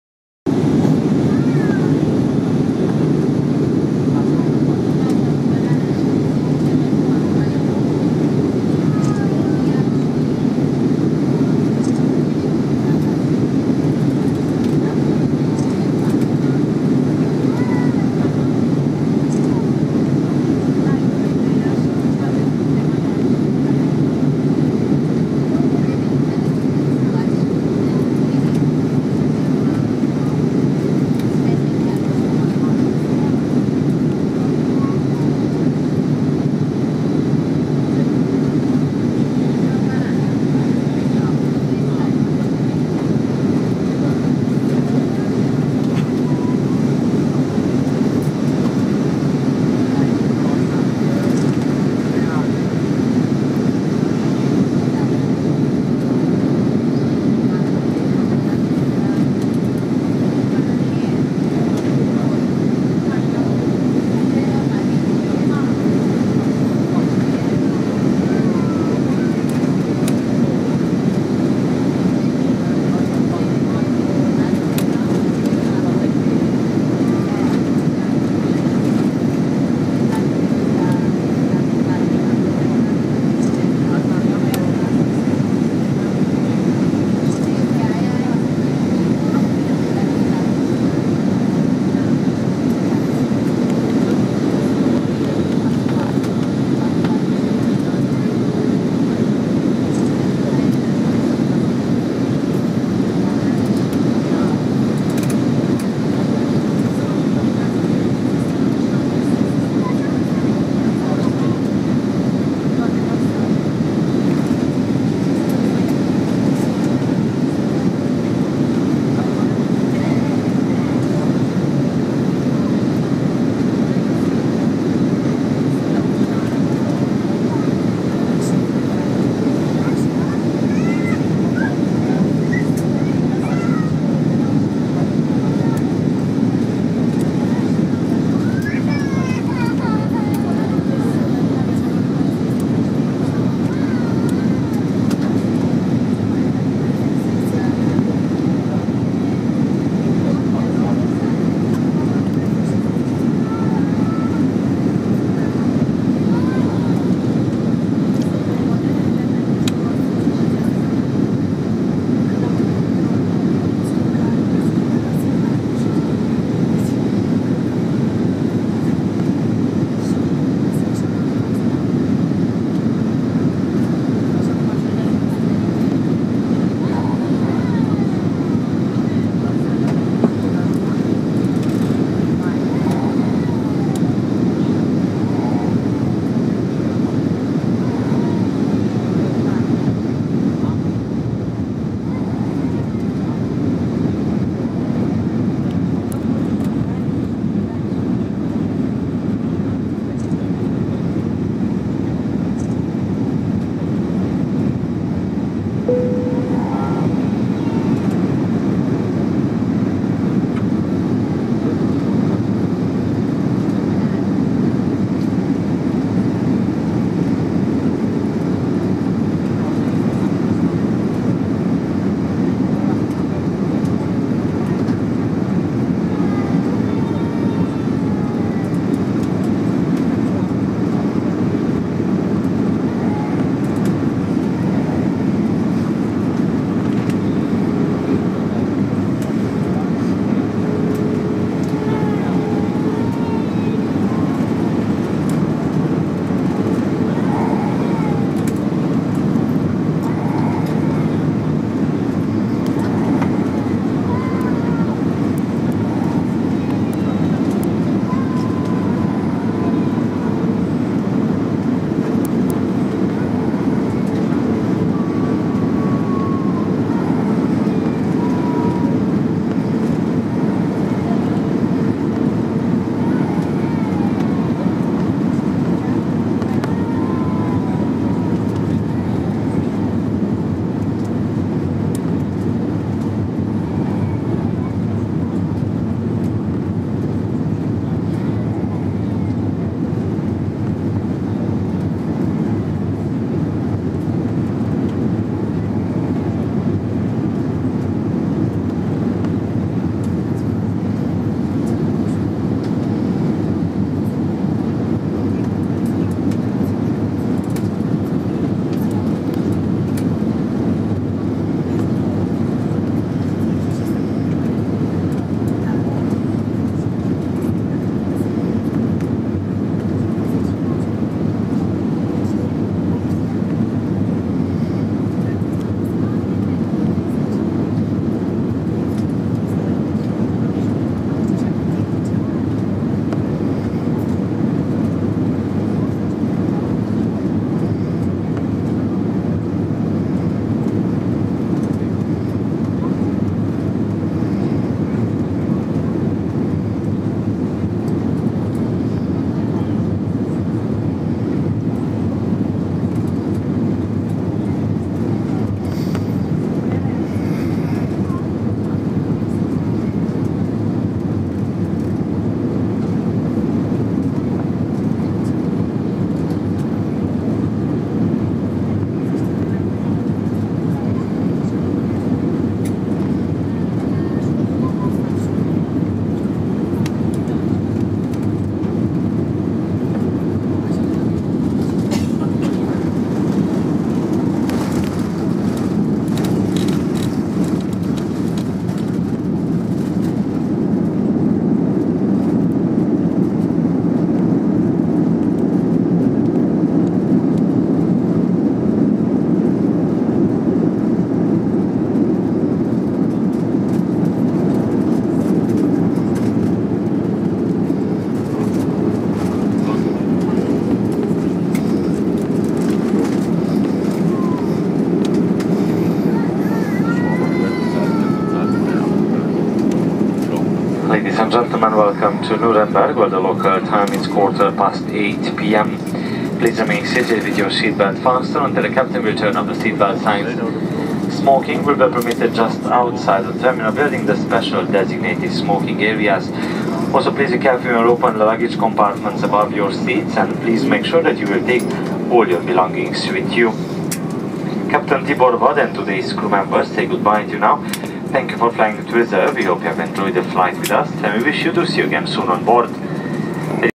Nürnberg (NBG), Final descent and landing in Nürnberg RW28, Airbus A320, HA LYI, W6 3369
Cluj Napoca (CLJ) - Nürnberg (NBG), Final descent and landing in Nürunberg on Runway 28, 2015|08|02, Airbus A 320, HA-LYI, W6 3369